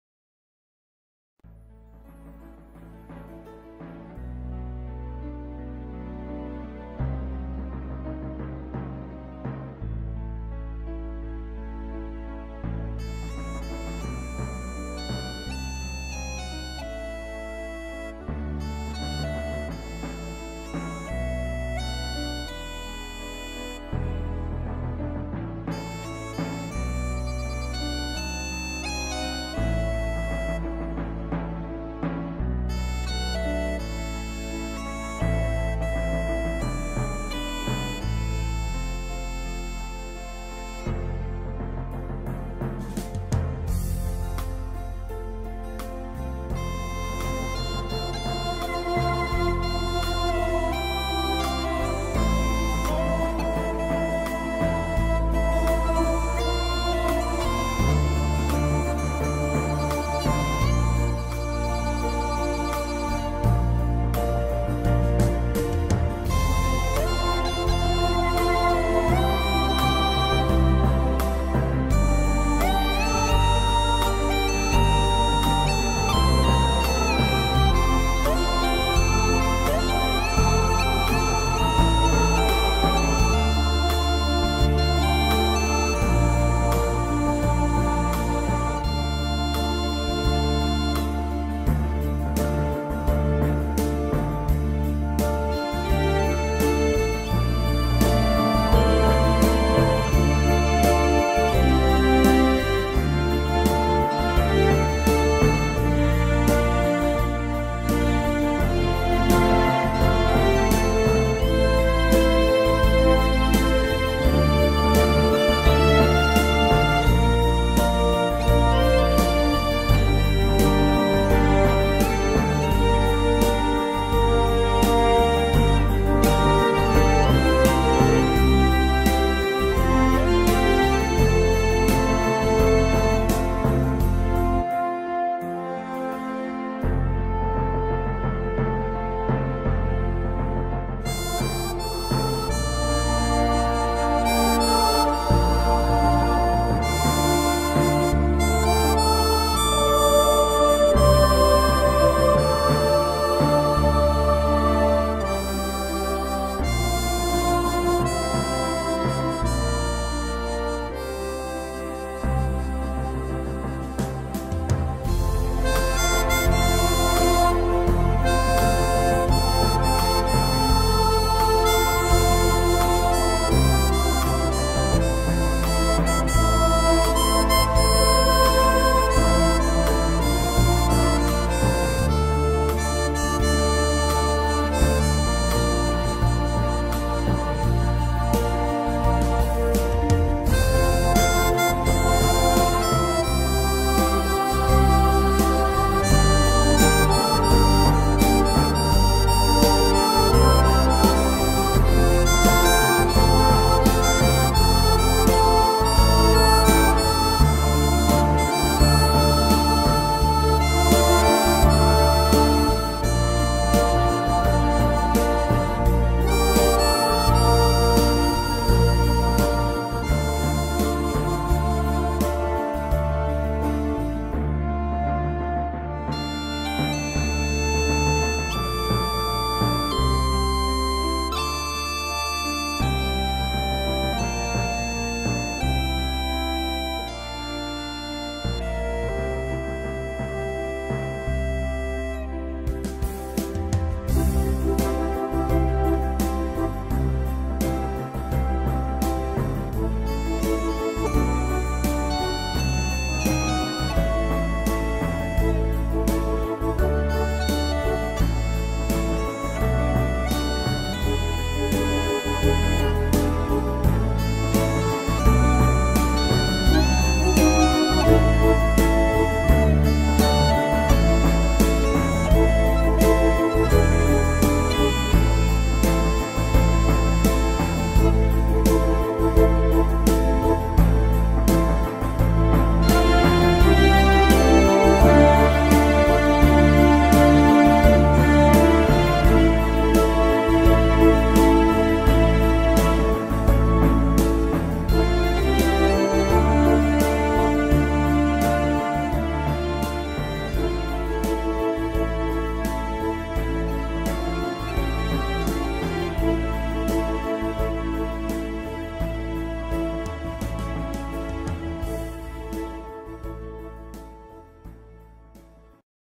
A la découverte du terril de Raismes
En balade avec mon ami sur le second terril de Raisme's (59) je vous invite a nous accompagner sur une musique de mon ami Christian Abric que vous pouvez retrouver